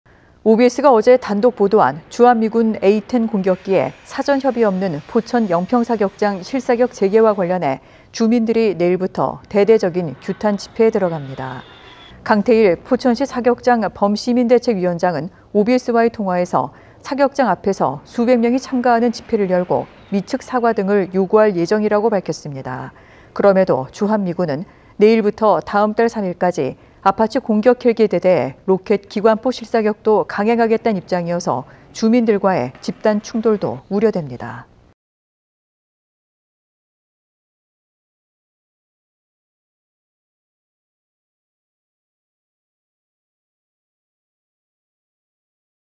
[단독] 포천 영평사격장 실사격 맞서 내일부터 규탄 집회
OBS가 어제 단독보도한
'주한미군 A-10 공격기의
사전 협의 없는 포천 영평사격장
실사격 재개'와 관련해
주민들이 내일부터
대대적인 규탄 집회에 들어갑니다.

강태일 포천시 사격장 범시민대책위원장은
OBS와의 통화에서 "사격장 앞에서
수백 명이 참가하는 집회를 열고,
미측 사과 등을 요구할 예정"이라고
밝혔습니다.

그럼에도 주한미군은
"내일부터 다음달 3일까지
아파치 공격헬기 대대의 로켓, 기관포 실사격도
강행하겠다"는 입장이어서
주민들과의 집단 충돌도 우려됩니다.